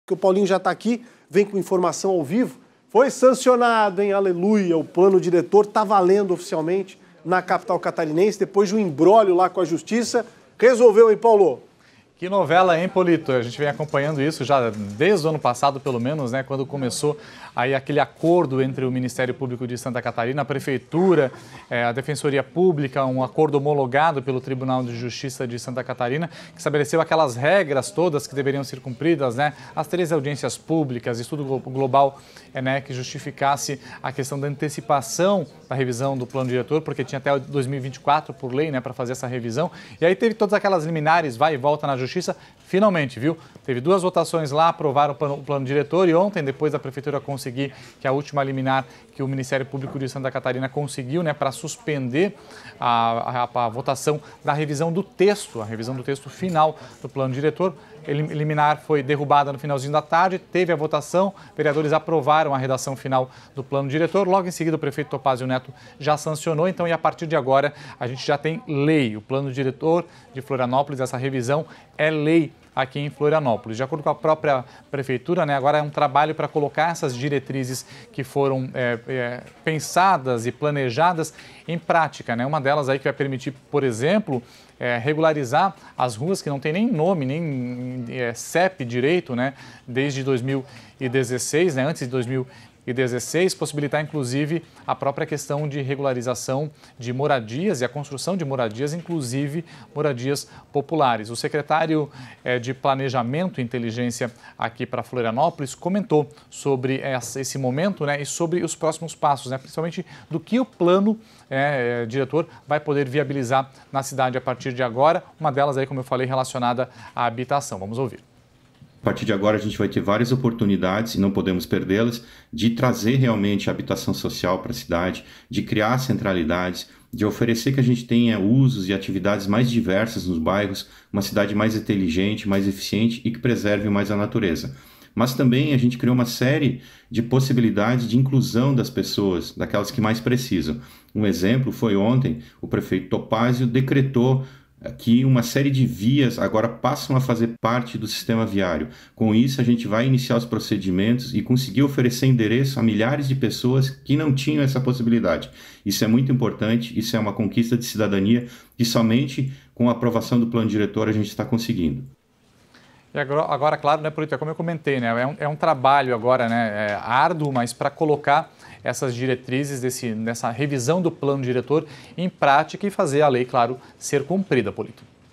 Prefeito sanciona projeto do plano diretor de Florianópolis
Acompanhe todos os detalhes dessa situação com o repórter Paulo Mueller.